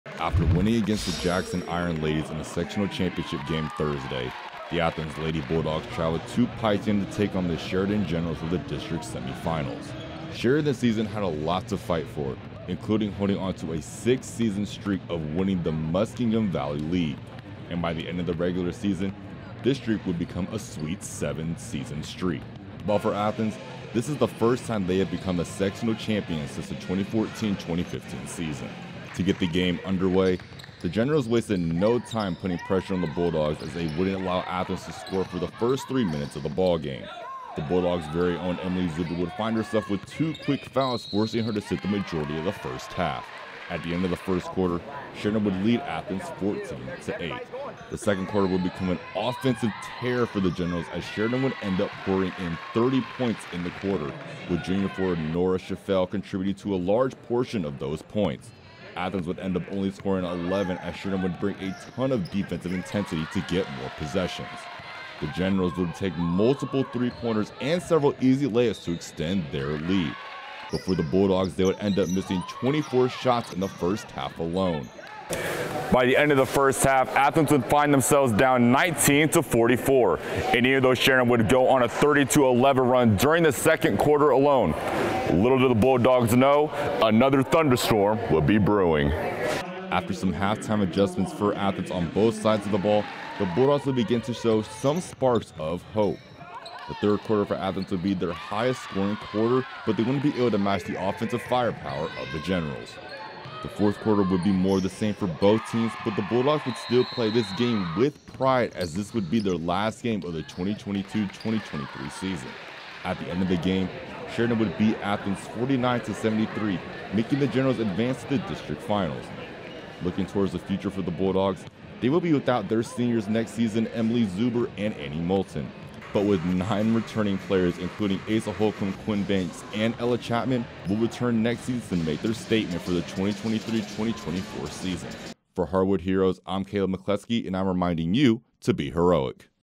Athens Lady Bulldogs at Sheridan Lady Generals - Video Recaps
Our Caleb McCleskey breaks down the matchup between the Athens Lady Bulldogs and Sheridan Lady Generals on February 20th, 2023